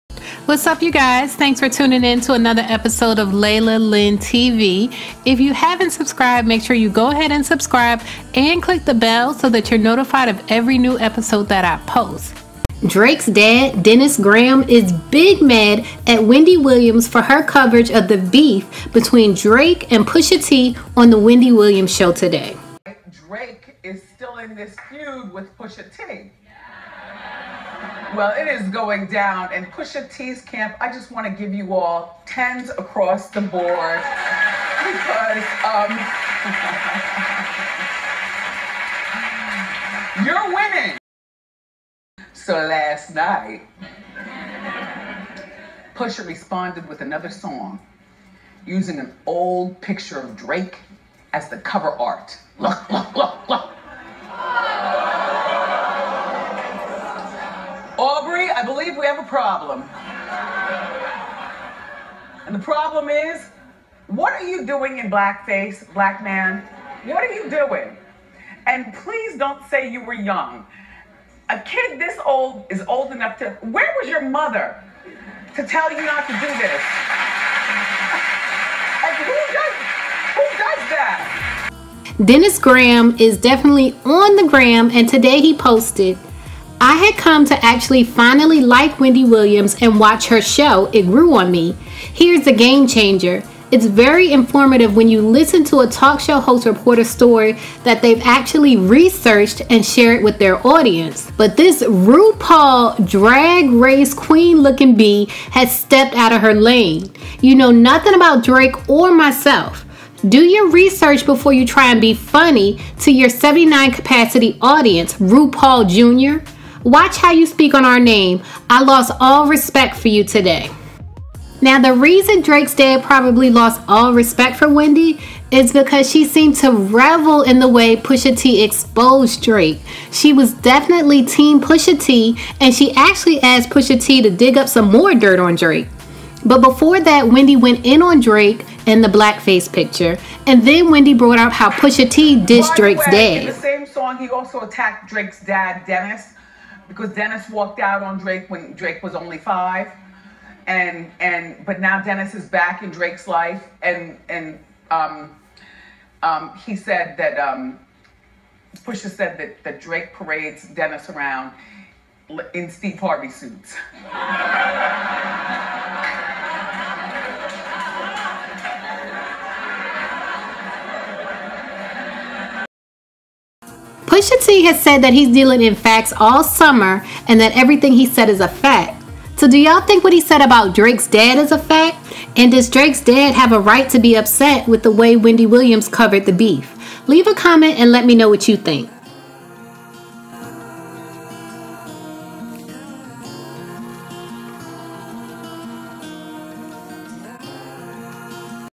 Drake's Dad Claps Back At Wendy Williams! He Calls Her RUPAUL After She Dissed Drake On Her Show
Drake's Dad, Dennis Graham, is BIG mad at Wendy Williams for her coverage of the beef between Drake and Pusha T on the Wendy Williams show today.
Drake's Dad got on Instagram and let Wendy Williams know EXACTLY how he felt!

Quality hair extensions with a 30 day money back guarantee

A D V E R T I S E  W I T H  M E